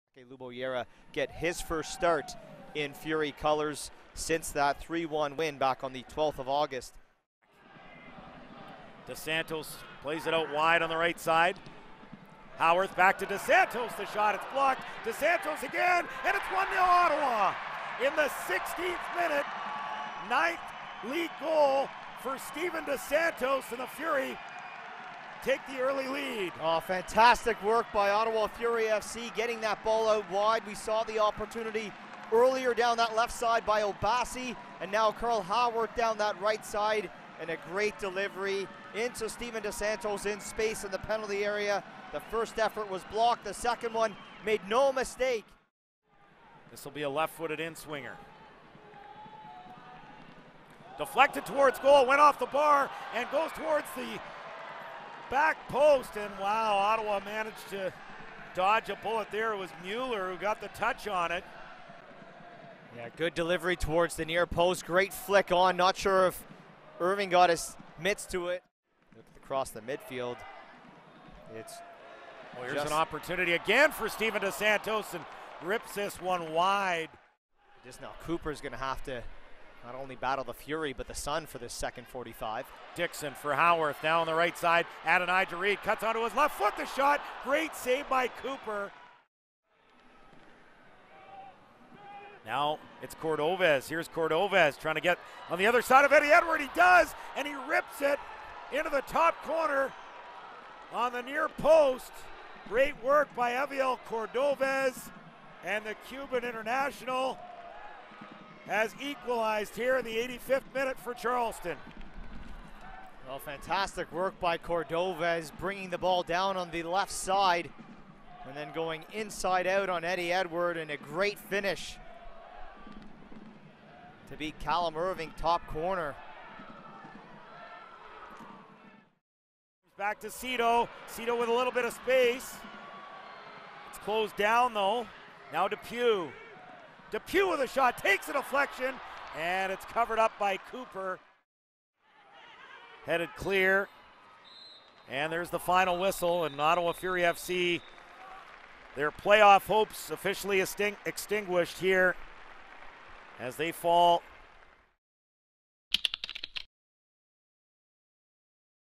HIGHLIGHTS: OTTvCHS 10/1/17
After Charleston went down to 10 men, Heviel Cordoves notched an equalizer in the 85th minute to secure a place in the playoffs for the Battery.